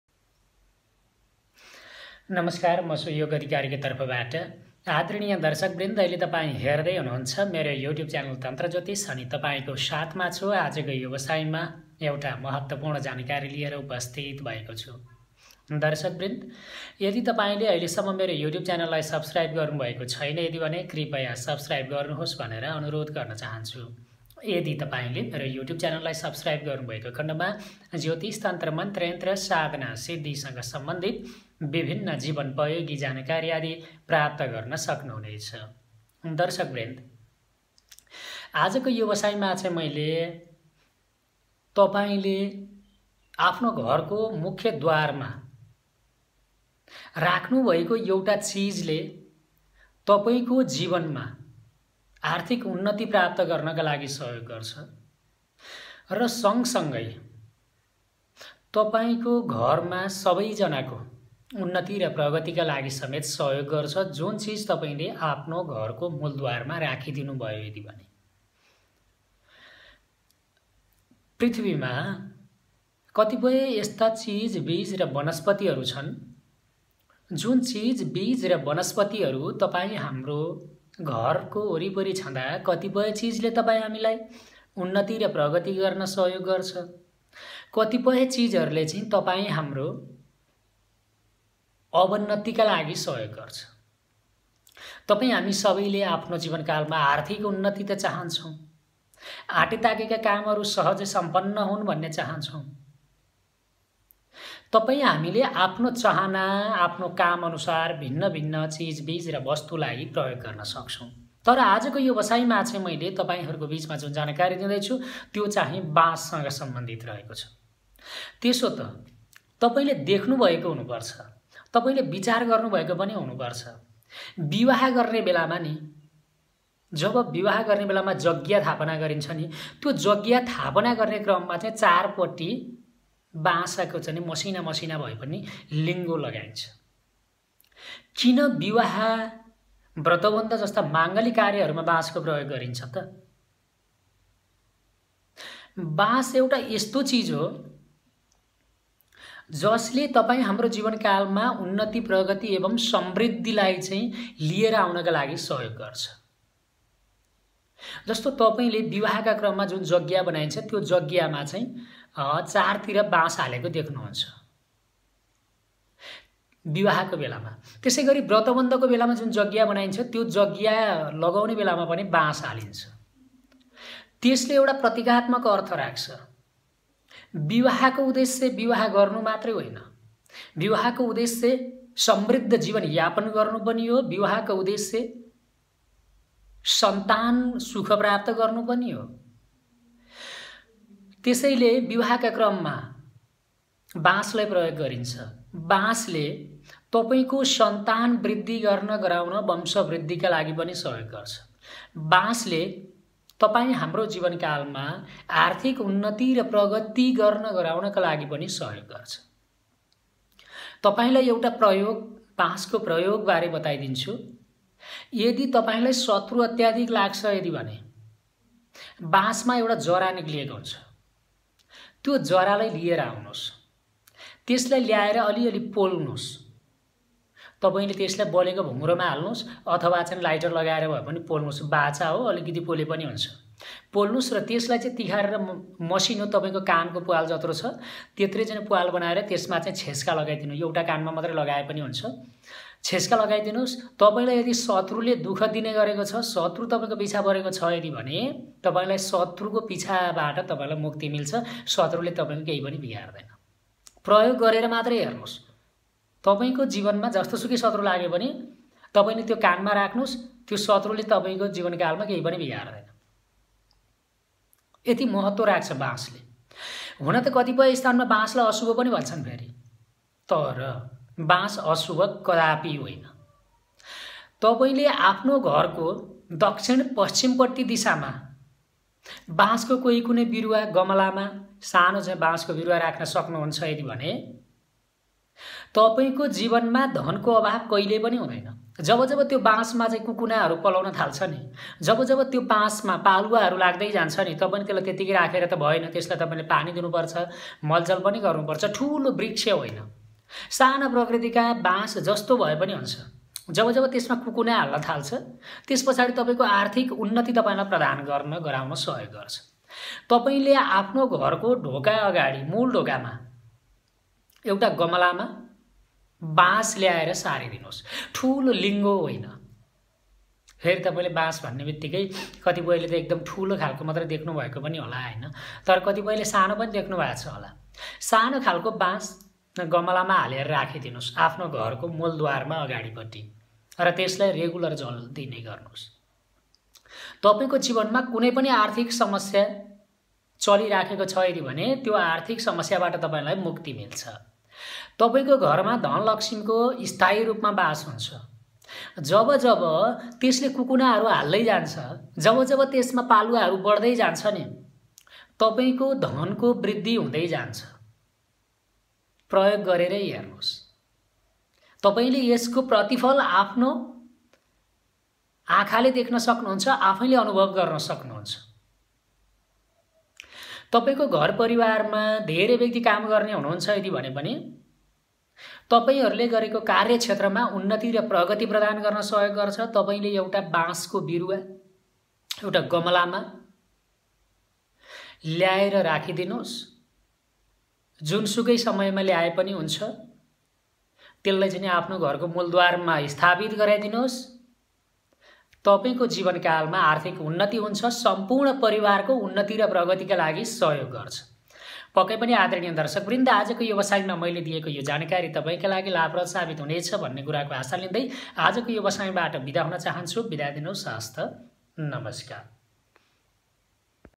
घरको मुलद्वारमा एक चिज राखिदिनुस , सम्पुर्ण परिवारको उन्नति हुन्छ||tantrajyotish||astro tips2021
साथिहरु tantrajyotish को यो सामाग्री लाभप्रद साबित हुने छ भन्नेकुराको आसा लिएको छु|